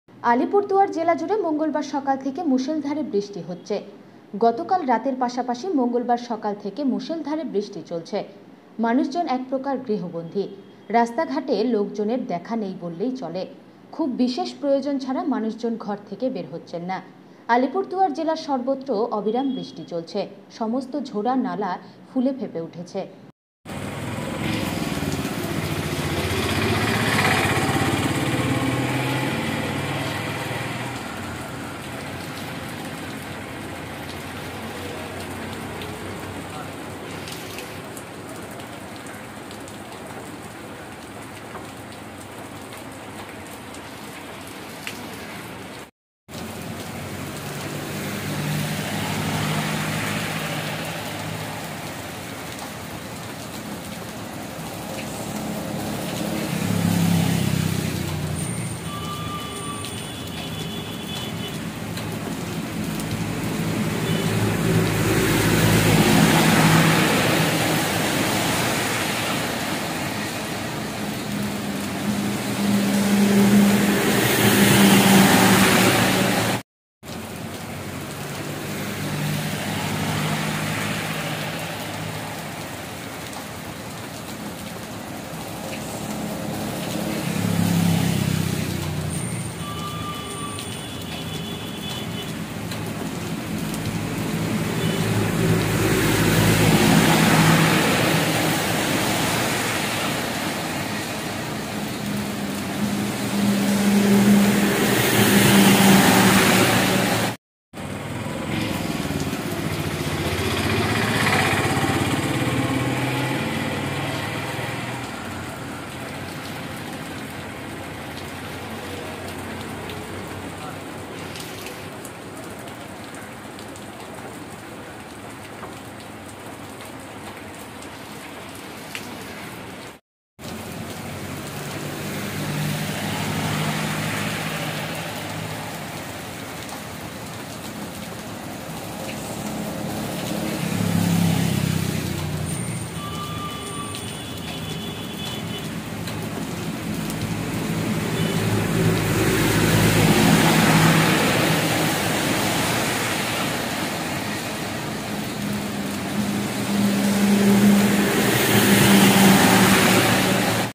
আলিপুরদুয়ার জেলা জুড়ে চলছে মুষলধারে বৃষ্টিপাত।
আলিপুরদুয়ার জেলা জুড়ে মঙ্গলবার সকাল থেকে মুষলধারে বৃষ্টি হচ্ছে। গতকাল রাতভর বৃষ্টি হয়েছে এবং মঙ্গলবার সকাল থেকে মুষলধারে বৃষ্টি হয়ে চলছে। মানুষ জন একপ্রকার গৃহবন্দী । সড়কে লোকজনের দেখা নেই বললেই চলে। খুব বিশেষ প্রয়োজন ছাড়া মানুষ জন ঘর থেকে বের হচ্ছেনা।আলিপুরদুয়ার জেলার সর্বত্র অবিরাম বৃষ্টি হয়ে চলছে। সমস্ত ঝোরা, নালা ফুলে ফেঁপে উঠেছে।